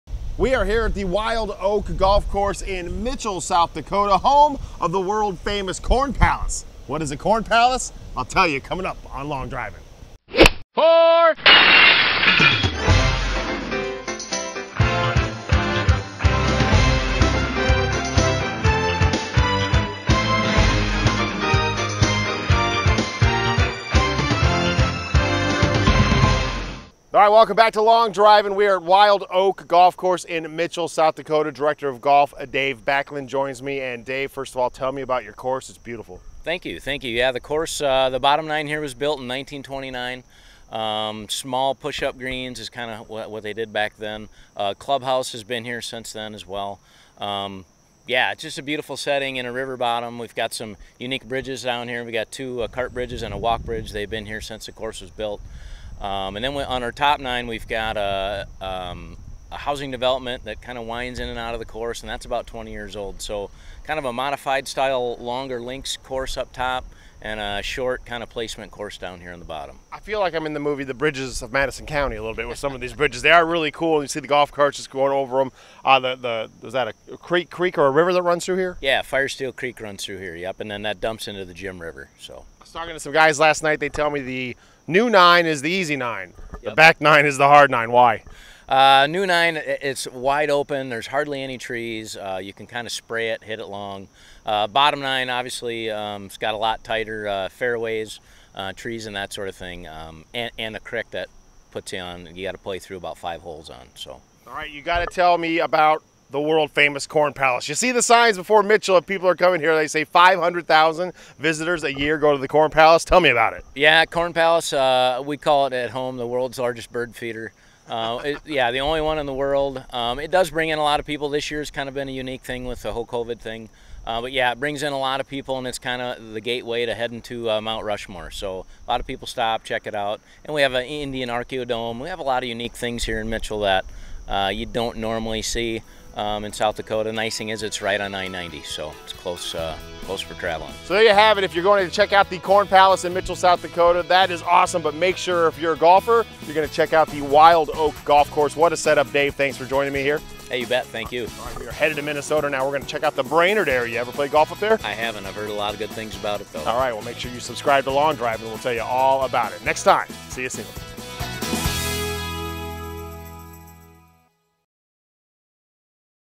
The Bridges of Corn Palace County
The next time you are traveling through the state of South Dakota and you see signs for "The World's Only Corn Palace" you will want to stop in the city of Mitchell.

Randy discovers a golf course tucked away on in a river valley is the Wild Oak, a course that's nearly 100 years old with amazing bridges from a century ago.